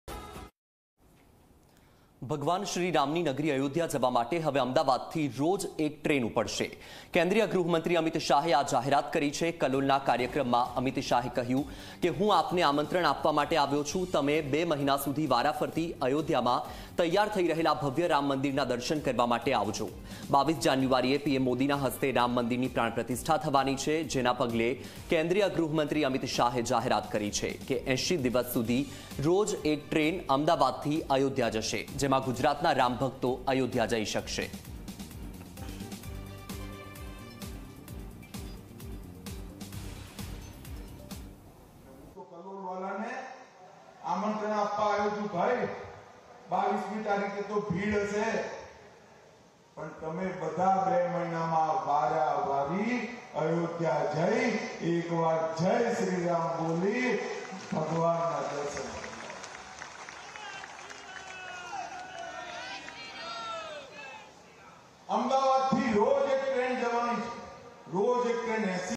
Ayodhya Ram Mandir: ભગવાન શ્રી રામની નગરી અયોધ્યા જવા માટે હવે અમદાવાદથી રોજ એક ટ્રેન ઉપડશે....
Ayodhya Ram Mandir: ભગવાન શ્રી રામની નગરી અયોધ્યા જવા માટે હવે અમદાવાદથી રોજ એક ટ્રેન ઉપડશે, કેન્દ્રીય ગૃહમંત્રી અમિત શાહે આ જાહેરાત કરી

ભગવાન શ્રી રામની નગરી અયોધ્યા જવા માટે હવે અમદાવાદથી રોજ એક ટ્રેન ઉપડશે. કેન્દ્રીય ગૃહમંત્રી અમિત શાહે આ જાહેરાત કરી છે. કલોલના કાર્યક્રમમાં અમિત શાહે કહ્યું કે હું આપને આમંત્રણ આપવા આવ્યો છું. તમે 2 મહિના સુધી વારાફરતી અયોધ્યામાં તૈયાર થઈ રહેલા ભવ્ય રામ મંદિરનાં દર્શન કરવા માટે આવજો. 22 જાન્યુઆરીએ પીએમ મોદીના હસ્તે રામ મંદિરની પ્રાણ પ્રતિષ્ઠા થવાની છે. જેના પગલે કેન્દ્રીય ગૃહમંત્રી અમિત શાહે જાહેરાત કરી છે કે 80 દિવસ સુધી રોજ એક ટ્રેન અમદાવાદથી અયોધ્યા જશે. જેમાં ગુજરાતના રામ ભક્તો અયોધ્યા જઈ શકશે.